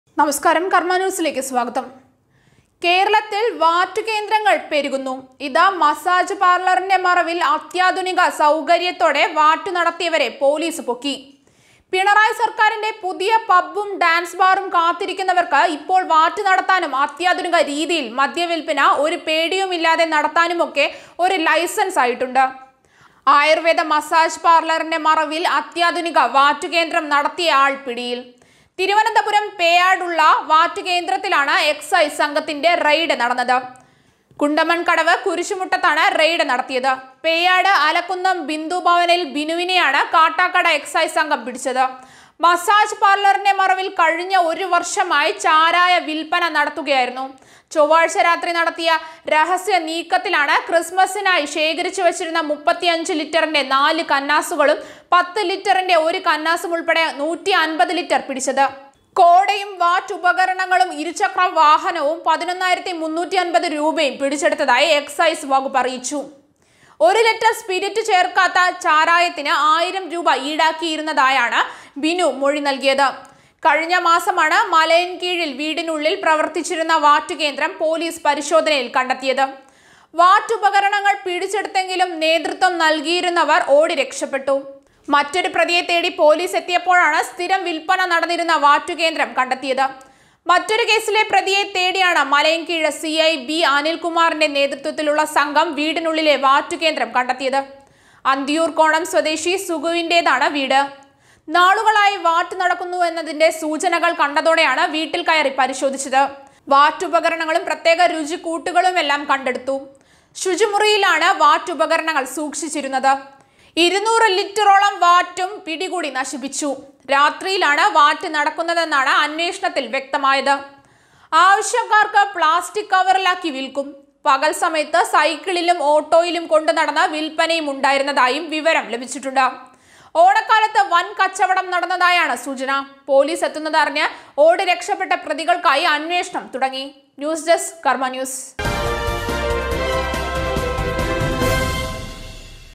മസാജ് പാര്‍ലറില്‍ അത്യാധുനിക വാറ്റുകേന്ദ്രം; പോലീസ് പൊക്കിയതിങ്ങനെ| karma news
ആയുര്‍വേദ മസാജ് പാര്‍ലറിന്റെ മറവില്‍ അത്യാധുനിക വാറ്റുകേന്ദ്രം . തിരുവനന്തപുരം പേയാടുള്ള വാറ്റുകേന്ദ്രത്തിലെ മുതലാളിയേയും തൊഴിലാളിയെയുമൊക്കെ  എക്‌സൈസ് കൈയോടെ പൊക്കി